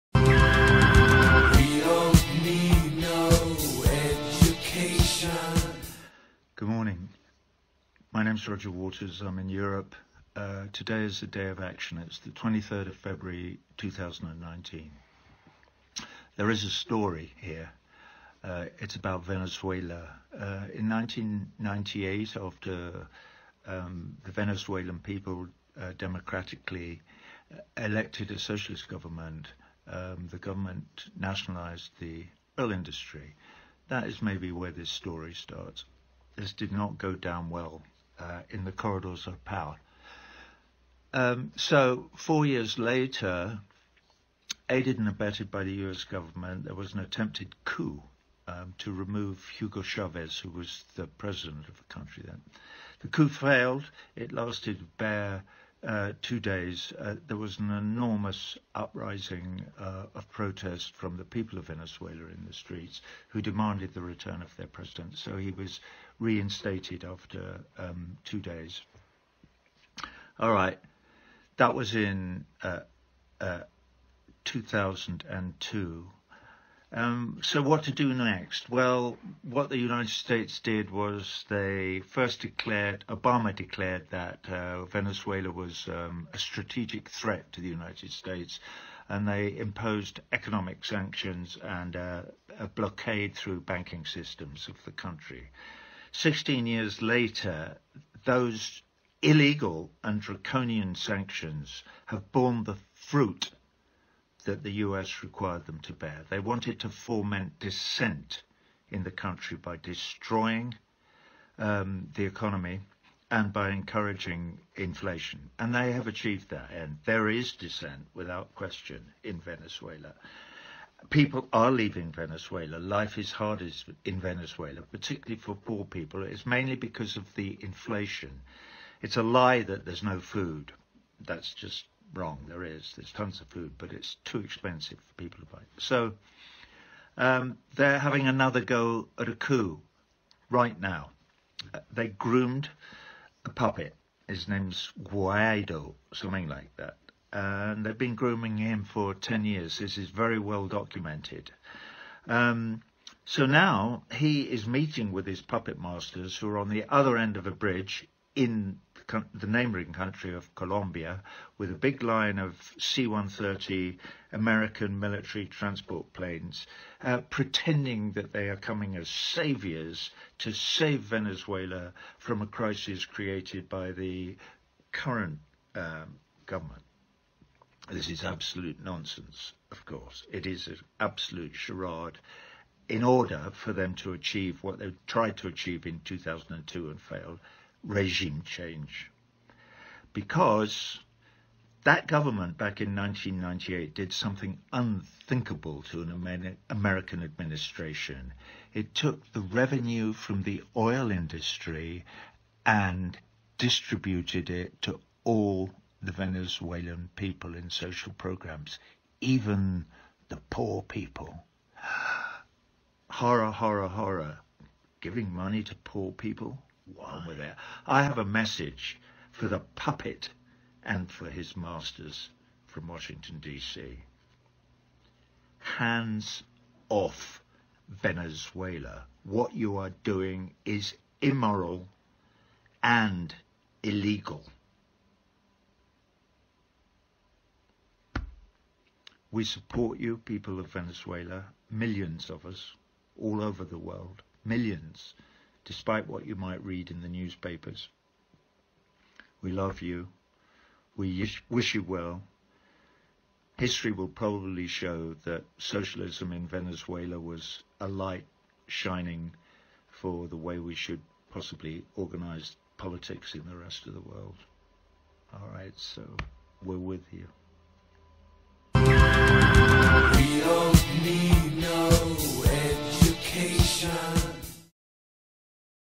Roger Water: NO alla guerra al Venezuela
Buon giorno. Mi chiamo Roger Waters. Sono in Europa e oggi è il giorno dell’azione: 23 febbraio 2019 C’è una storia da raccontare. E’ a proposito del Venezuela.

Nel 1998 il popolo ha democraticamente eletto un governo
socialista che ha nazionalizzato l’industria petrolifera
Ed è qui che forse, la storia parte.

Questo non è stato preso bene nei corridoi del potere.
E così quattro anni dopo, orchestrato dal governo degli Stati Uniti,
c’è stato un tentativo di colpo di stato per rimuovere

Hugo Chavez che era
il Presidente del Paese.

Il colpo di stato è fallito. E’ durato due giorni.
C’è stata una sollevazione popolare nelle strade
che ha chiesto il ritorno del Presidente.

E due giorni dopo
è tornato al potere.

Questo, nel 2002, e quindi che fare dopo? Beh quello
che gli Stati Uniti hanno fatto.  Obama ha dichiarato
che il Venezuela era una minaccia strategica

per gli Stati Uniti e hanno imposto sanzioni economiche e
un blocco attraverso il sistema bancario del Paese.

16 anni dopo queste illegali e draconiane sanzioni
hanno ottenuto quello che si prefiggevano.

Fomentare il dissenso nel paese,
distruggendo l’economia e incoraggiando inflazione
E ci sono riusciti.

C’è dissenso, senza dubbio, In Venezuela.
Le persone stanno lasciando il Venezuela. La vita è
difficile in Venezuela, in particolare per i poveri

Questo principalmente per l’inflazione. Ma è una
bugia che non ci sia cibo. ci sono tonnellate di cibo
Ma è troppo costoso per i poveri.

Quindi, siamo nel pieno di un altro colpo di stato,
proprio in questo momento, per mettere al potere un
fantoccio: il suo nome è Guaidò o qualcosa di simile.

E ci stanno lavorando da 10 anni. E’ tutto ben
documentato e ora si sta incontrando con i suoi
padroni che sono ora dall’altra parte del ponte in Colombia,

Con un una lunga serie di C130 americani. Facendo finta di
essere arrivati per salvare il Venezuela da una crisi creata
dal governo. E’ un non senso assoluto naturalmente.

È una pagliacciata che serve a realizzare l’obiettivo del 2002
Vale a dire destituire un governo che ha attuato qualcosa di
impensabile per un’amministrazione americana.

Ha trattenuto le rendite dell’industria petrolifera
e le ha redistribuite a tutta la popolazione venezuelana
in programmi sociali.

Anche i poveri… ha dato soldi ai poveri. Orrore!
Si perché è questo il nodo centrale di quello che vi
sto parlando oggi.

Ho un messaggio per il fantoccio e per i suoi padroni
che sono a Washington DC.
Giù le mani dal Venezuela!

Quello che state facendo è immorale e illegale.
Vi sosteniamo, popolo del Venezuela,
in milioni, da tutto il mondo.

Milioni, nonostante quello che
potete leggere dai giorni,
vi amiamo.

Vi auguriamo ogni bene. La Storia probabilmente mostrerà che il
socialismo in Venezuela sia stata una luce per insegnarci il modo
in cui dovremmo organizzare la politica nel resto del mondo.